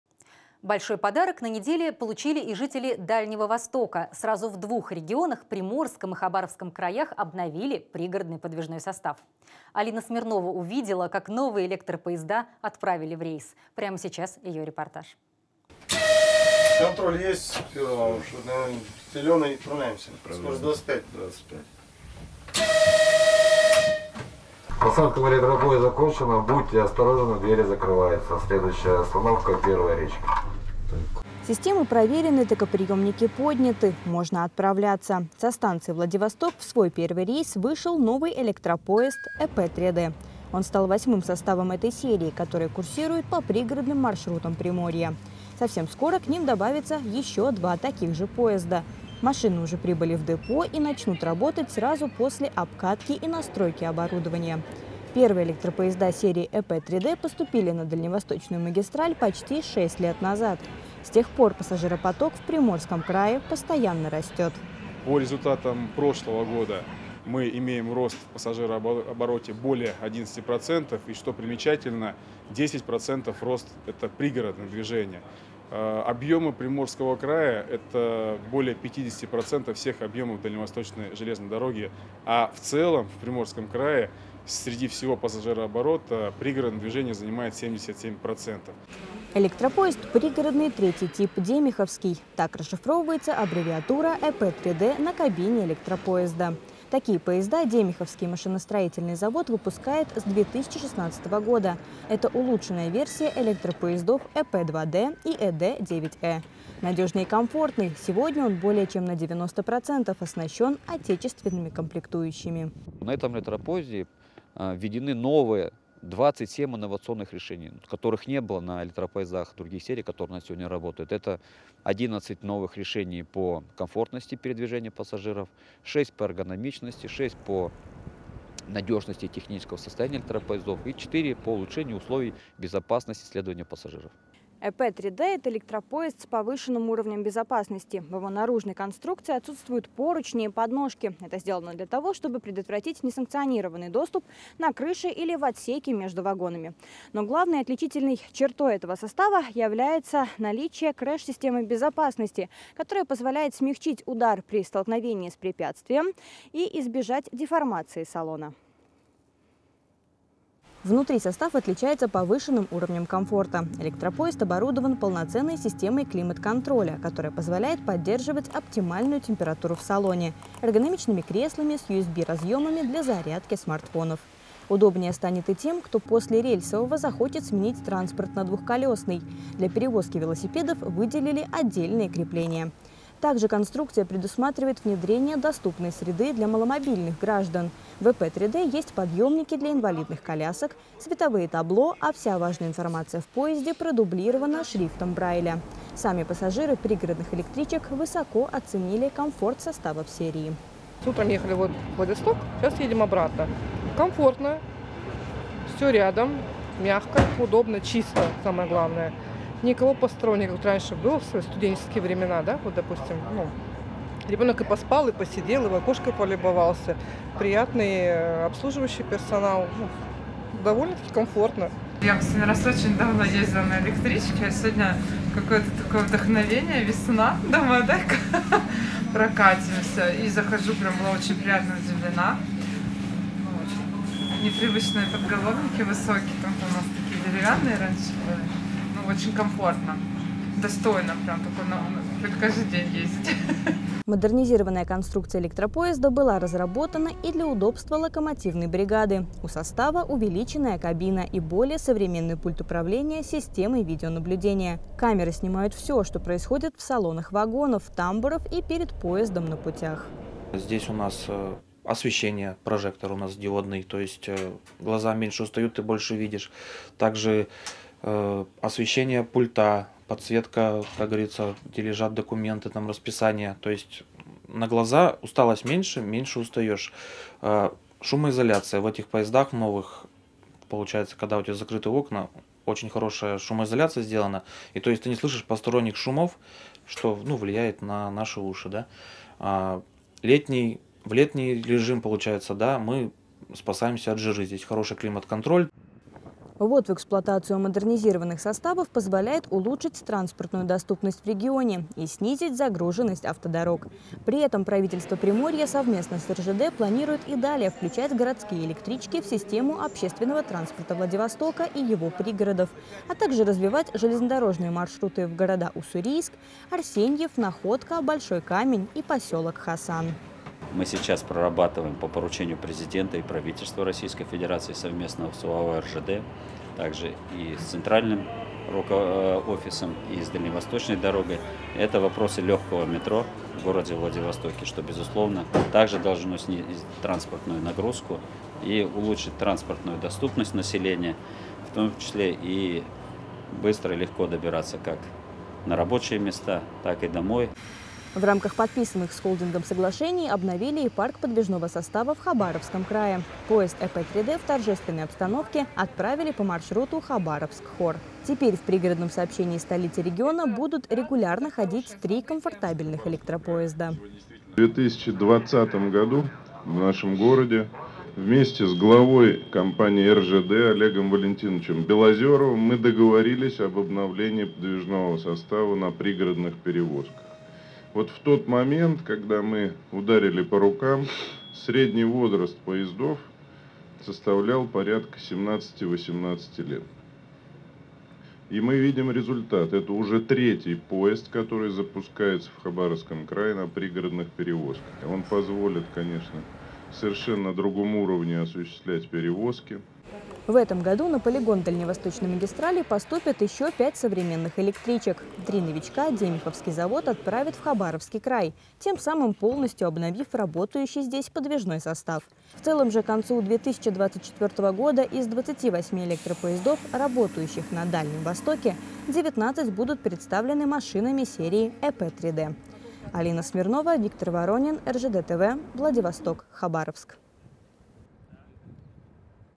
На ДВЖД запустили обновлённый электропоезд ЭПЗД || Итоги недели 10.03.2024
Большой подарок получили жители Дальнего Востока. Сразу в двух регионах – Приморском и Хабаровском краях обновился пригородный подвижной состав. В городах  запустили обновлённый электропоезд ЭПЗД, поезда позволяют совершать перевозки на высоком уровне с комфортом для пассажиров. В отправке первых рейсов приняла участие и наш корреспондент Алина Смирнова.